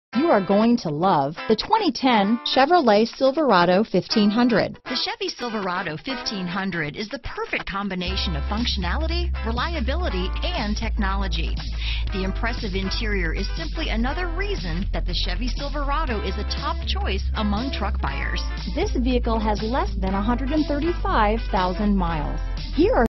2010 Chevrolet Silverado 1500 Lithia Springs, Douglasville, Powder Springs, Smyrna, Atlanta, GA 2123
Black Used 2010 Chevrolet Silverado 1500 available in Lithia Springs, Georgia at John Thornton Chevrolet. Servicing the Douglasville, Powder Springs, Smyrna, Atlanta, GA area.
2010 Chevrolet Silverado 1500 LT - Stock#: 21236A - VIN#: 3GCRKSE39AG214739